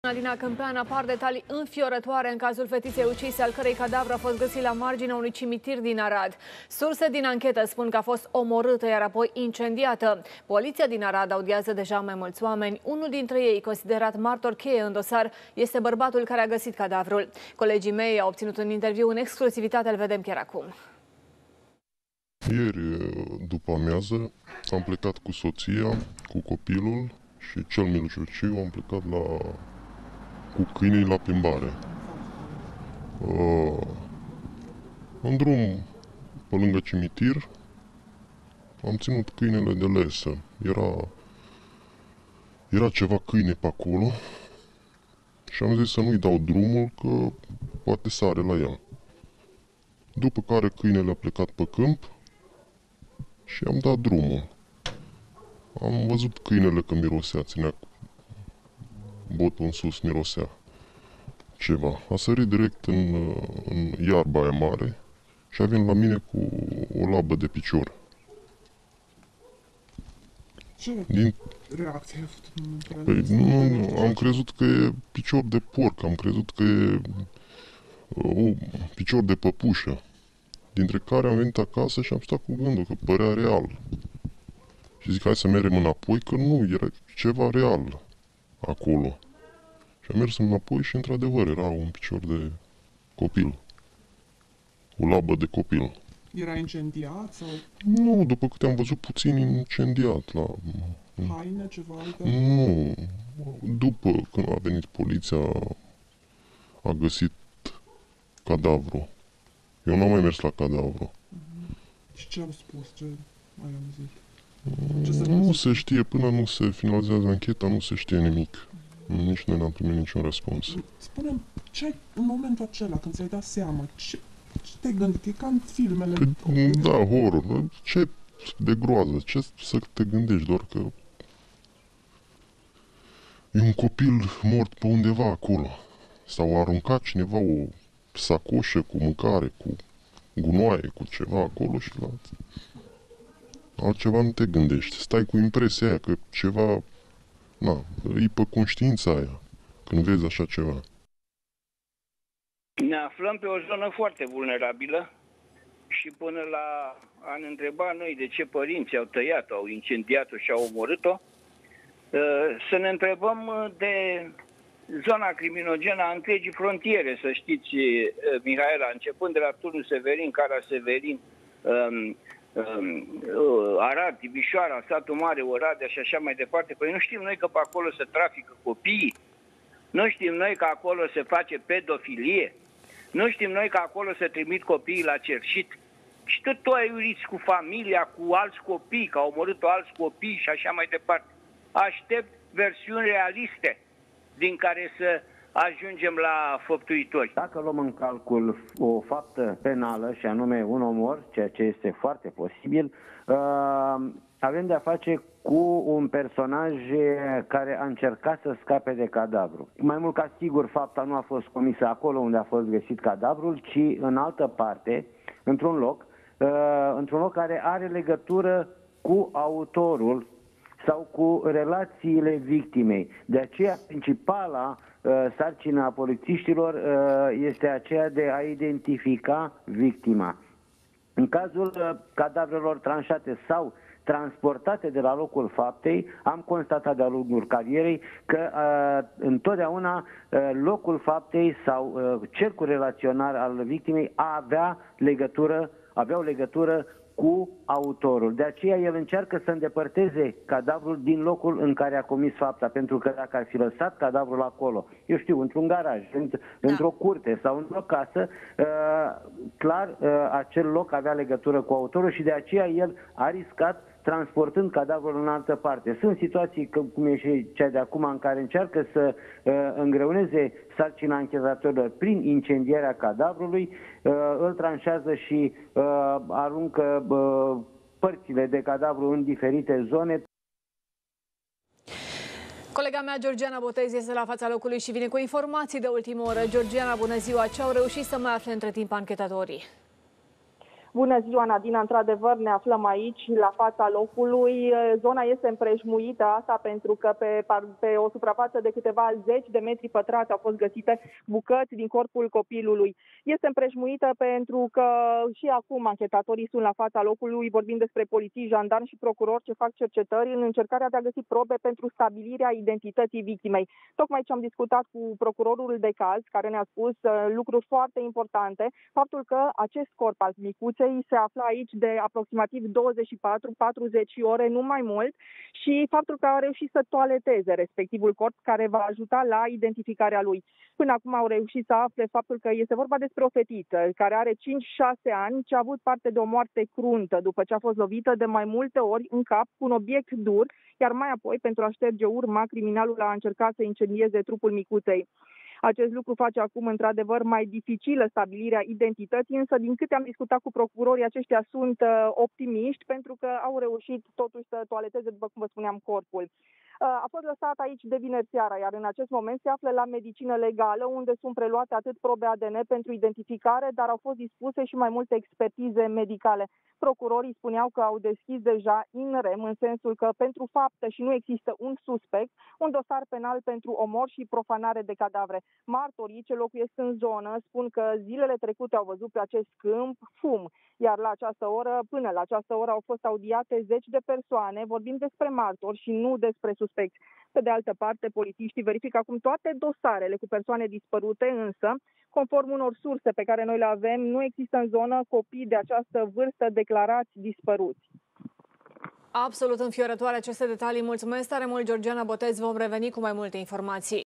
Bărbatul care a găsit cadavrul fetiței lângă cimitirul din Arad vorbește
Apar detalii înfiorătoare în cazul fetiței ucise, al cărei cadavru a fost găsit la marginea unui cimitir din Arad. Surse din anchetă spun că a fost omorâtă apoi incendiată. Poliția audiază deja mai mulți oameni.